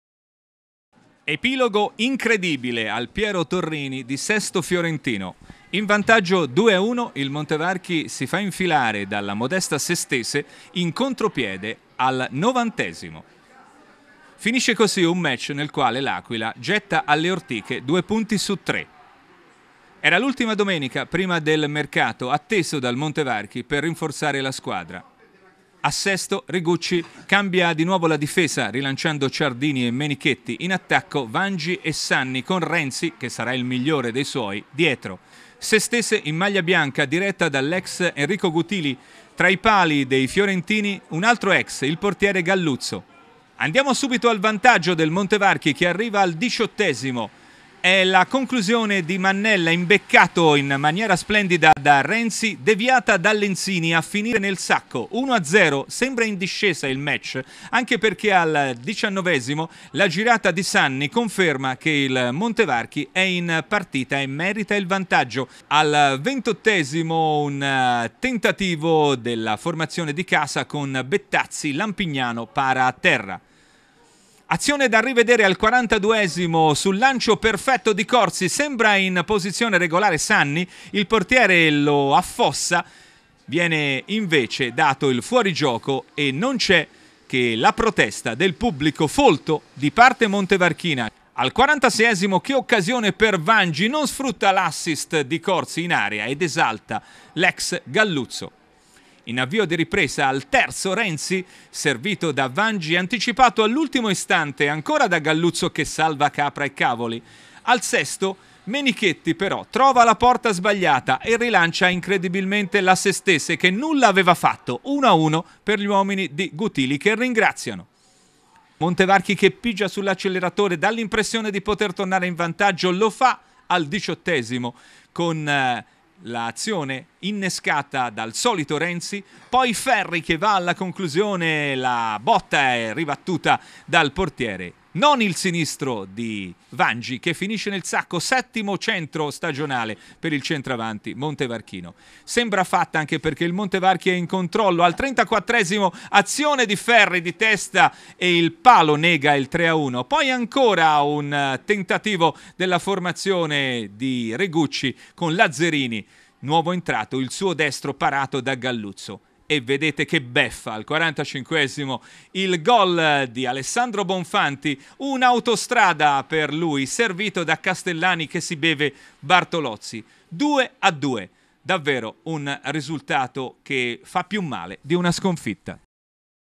sestese montevarchi 2 2
Incredibile beffa per il Montevarchi che spreca l'impossibile a Sesto Fiorentino e si fa raggiungere sul 2 a 2 al 91'.